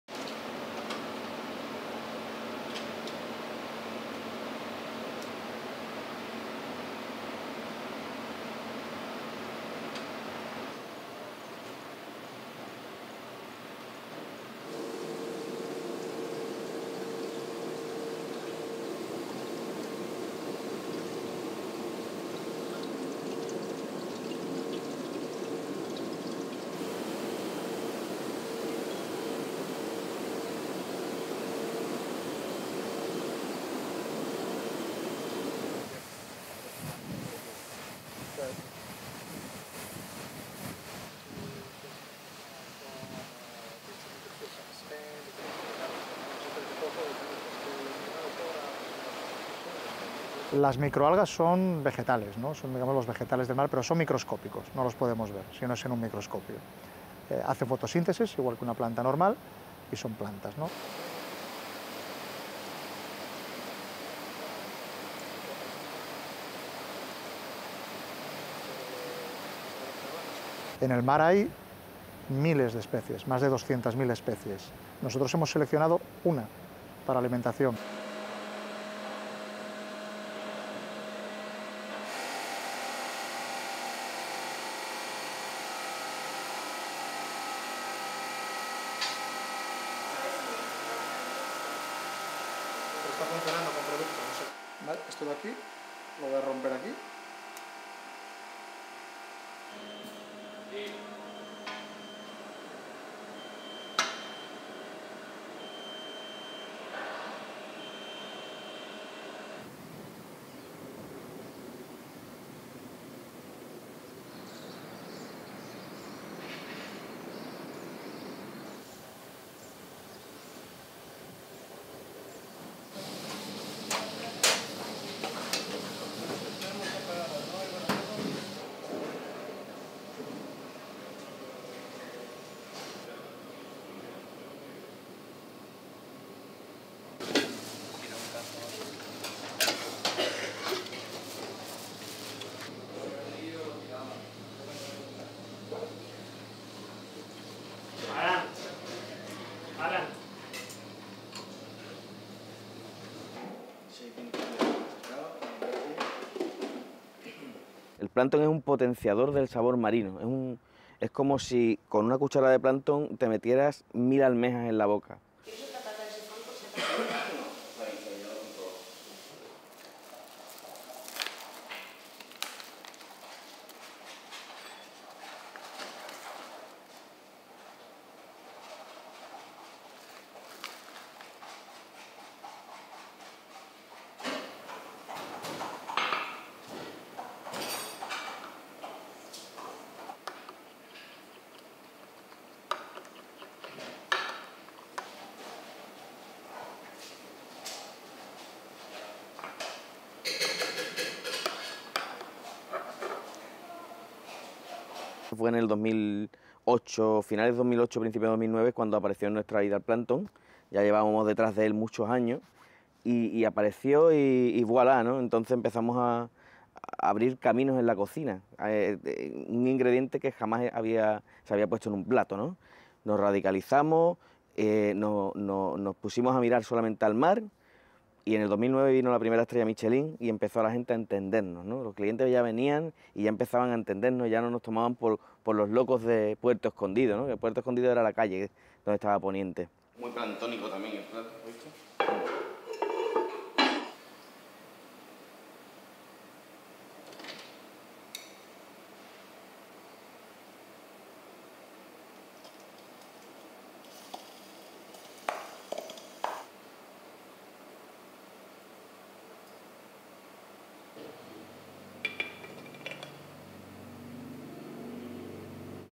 Microscopic algae is on the menu
(11 Aug 2016) FOR CLEAN VERSION SEE STORY NUMBER: 4049044

LEADIN:
The latest food trend is microscopic.
Tiny phytoplankton or microalgae are migrating from the sea to the lab before ending up on menus in high-end restaurants.

STORYLINE :
This lab worker is cultivating algae in sterile lab conditions. The bubbling mixture is destined for Michelin starred dinner tables in Southern Spain.
The conditions are controlled precisely because this represents big bucks.
The plankton sells for 3 Euros per gram, that's 3,000 Euros per kilo.
Fitoplancton Marino, the company that produces the microalgae, is based in El Puerto de Santa Maria.
Founded in 2002 they started producing and selling phytoplankton to feed fish larvae, which is still the main use of most of its products.
"Microalgae (phytoplankton) are vegetables, microscopic vegetables that live in the sea. We can't see them unless we use a microscope. They also photosynthesise like a normal plant" explains Carlos Unamunzaga, general manager and co-owner, Plancton Marino.
Fitoplancton Marino also produces microalgae for cosmetics, aquariums and fish larvae food.
The microalgae crops are placed in increasingly bigger sea-water containers and given enough carbon dioixide and light for photosynthesis.
The product, a species of phytoplankton, is sold in the form of dried powder for 3,000 euro per kilo, and used as a powerful sea-food flavour enhancer.
"You can find more than 200 thousand species (of phytoplankton) in the sea. We have selected just one species to create a food product " adds Unamunzaga.
Like saffron or chilli, just a little bit is enough.
In 2009, a local restaurant Aponiente joined forces with Fitoplancton Marino to test around three dozen species of microalgae for human consumption and they chose one called Tetraselmis chuii for its flavour.
"It's like putting a thousand clams in your mouth", says Juan Luis Fernandez, gastronomic director of Aponiente.
It's one of the around 1,000 haute-cuisine or high-end restaurants that use the ingredient in Spain and the European Union, according to Unamunzaga, others include
the two -Michelen starred Abac restaurant in Barcelona.
Fitoplancton Marino became the first microalgae production company to achieve the European Novel Food approval for the marine microalgae species Tetraselmis chuii.
European Novel Food approval is a compulsory regulation for any new human nutrition ingredient to be sold in the EU and not having a history of consumption in Europe before 1997.
Novel Food must be safe to eat and properly labelled to not mislead customers.
At least two more microalgae species are approved for consumption inside the EU – as they were used before 1997-, the Spirulina and Chlorella, but these are eaten mainly in tablets as dietary supplements.
Unamuzaga says human nutrition microalgae is a growing business representing 15 percent of his company's turnover last year.
Now diners' appetites have been whetted, the demand is growing.
"It was at the end of 2008 or the beginning of 2009 when plankton appeared in our lives. We had been tracking it many years, and then when it appeared. Voila, we started breaking new ground in cuisine. An ingredient that had never before been used in a dish. We radically (changed). We focused just on the sea (products). Then in 2009 came the first Michelin star, and people began to understand us." says Fernandez.
Fernandez, whose eatery was awarded two stars by the popular Michelin gourmet-guide, now uses plankton in many of his signature dishes.